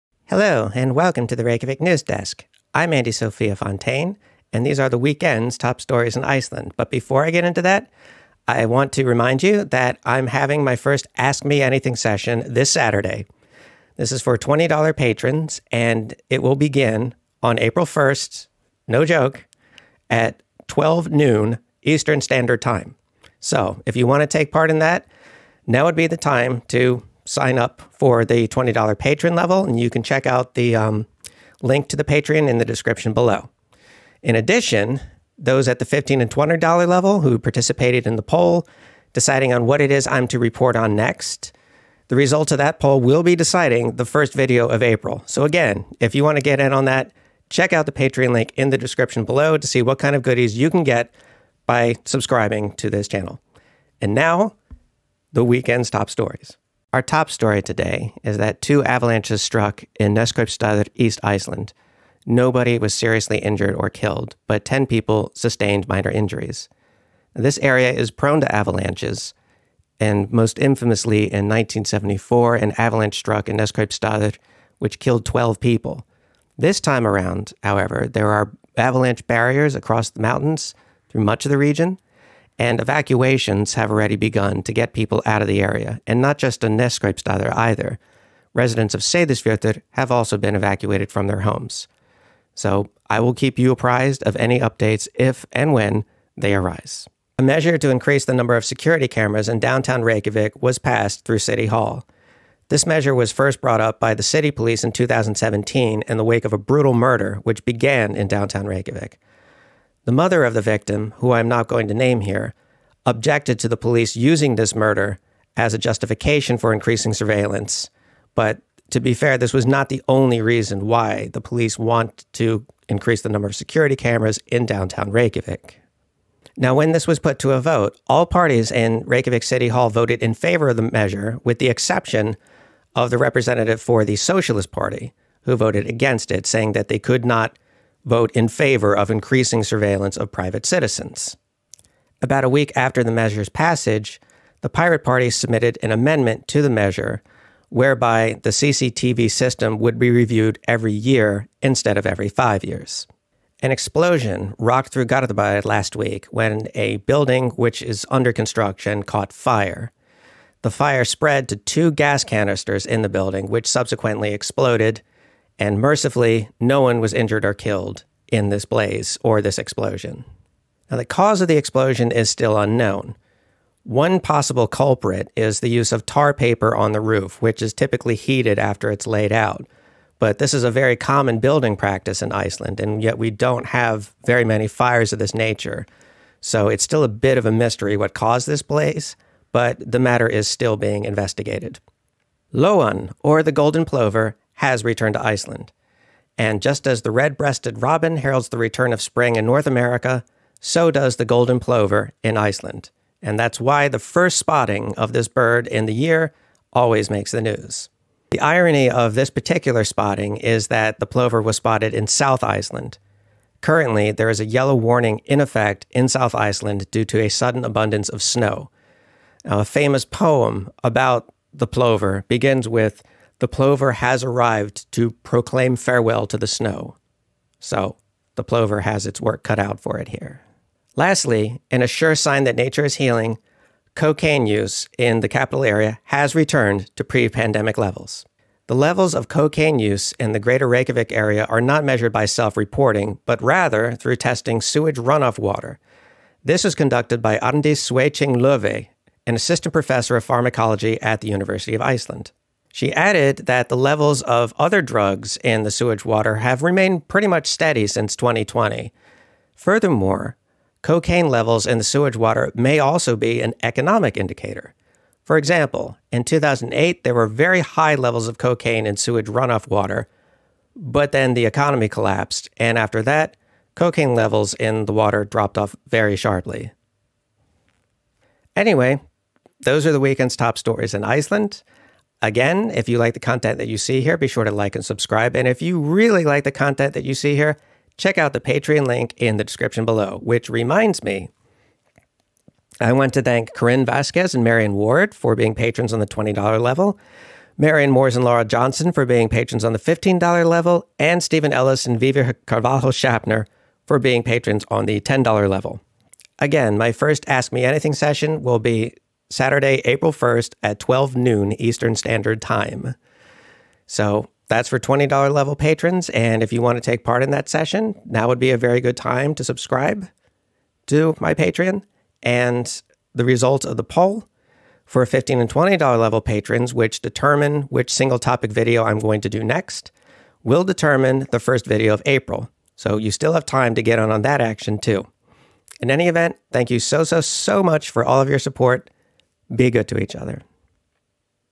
Avalanches In East Iceland
In this episode of Reykjavík Newsdesk, Andie Sophia talks about a pair of avalanches which struck East Iceland this morning, in addition to some other, less serious, stories.